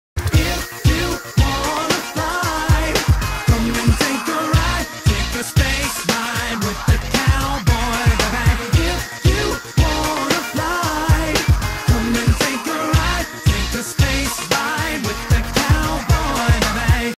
K0BE flying
i tried to fraps K0BE zerking, but i failed. so ill show the trial run i took